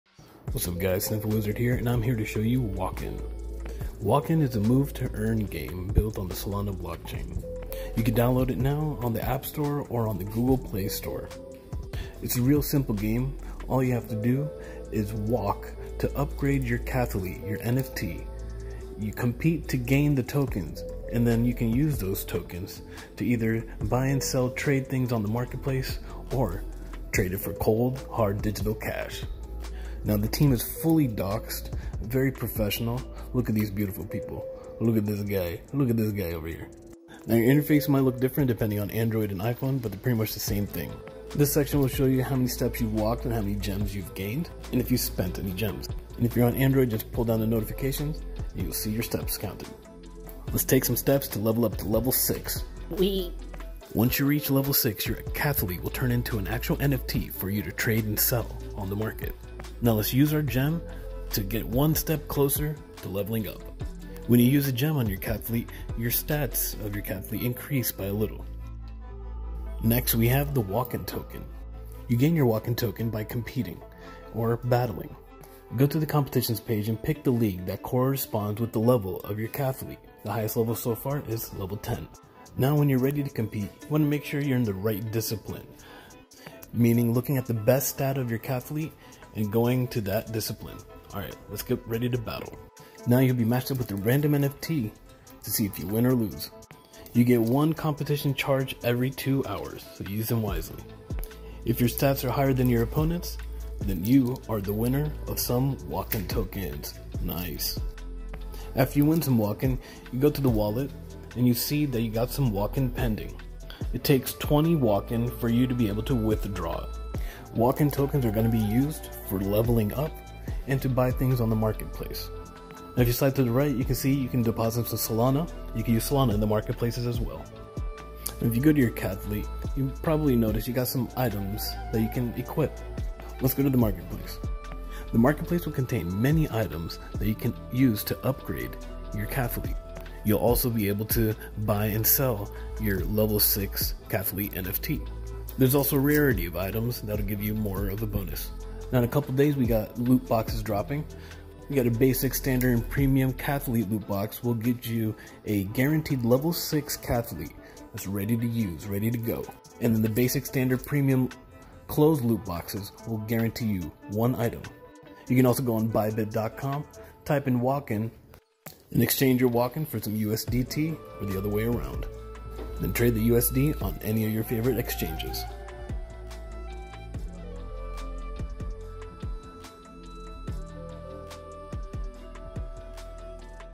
Walken - Make Money by Walking - Play to Earn game on Solana
Make money just by walking!
Walken.io is a move to earn game built on the Solana blockchain!
Walk to earn gems - use gems to level up your NFT - compete with other NFTs to win $WLKN - trade $WLKN for items to make your NFT stronger or exchange for USDT!